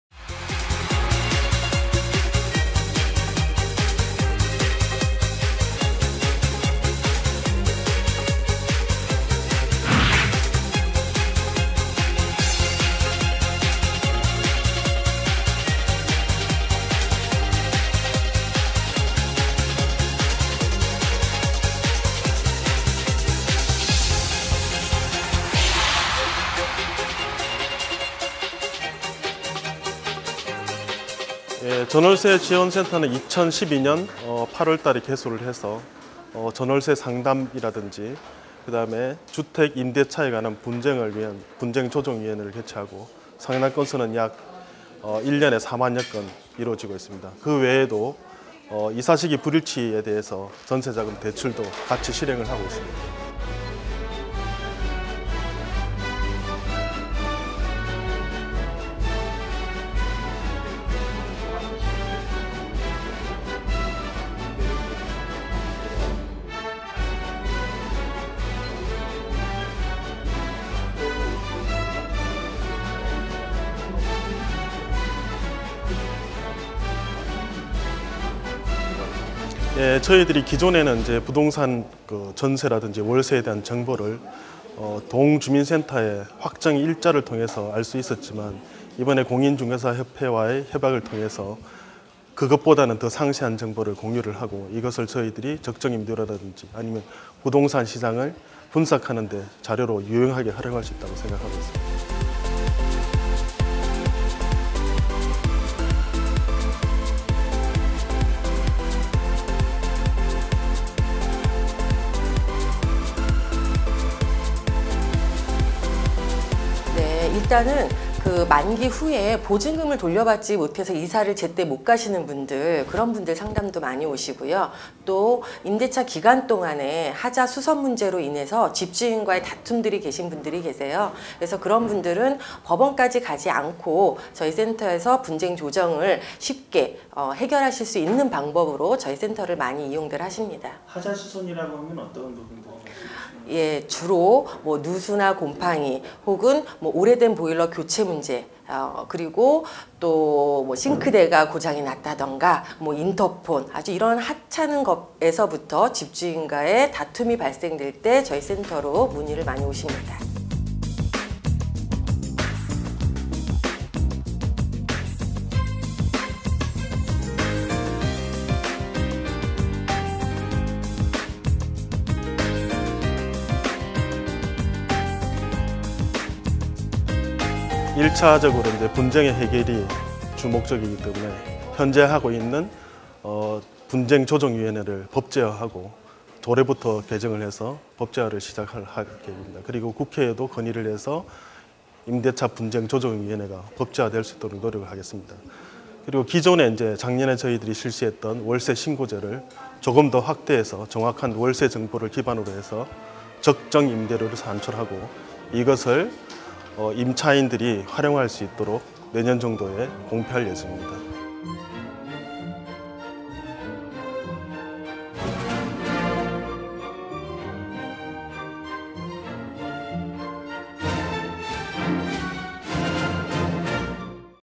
서울시전월세보증금지원센터 서민주거안정에 큰 도움
서울시 전월세보증금지원센터는 최근 심각한 전월세난에 대한 대책 수립과 이를 둘러싼 분쟁조정 등의 문제를 해결하기 위해 한국공인중개사협회와 업무협약을 채결했다. 이를 통해 임대차 선진화를 이뤄나갈 계획이다.